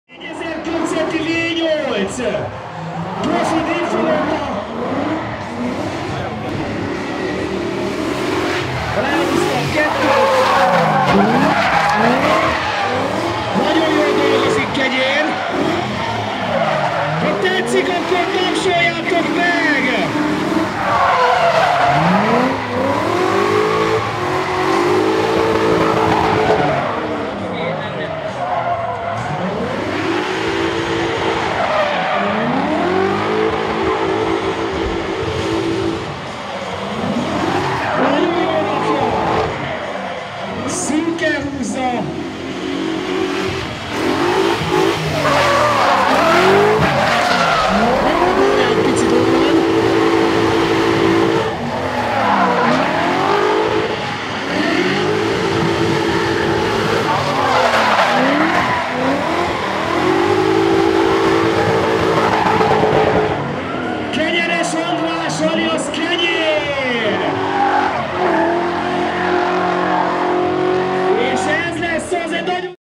Kenyeres András BMW Fest Soltvadkert 2014.
Kenyeres András bemutatója a soltvadkerti BMW Festen 2014.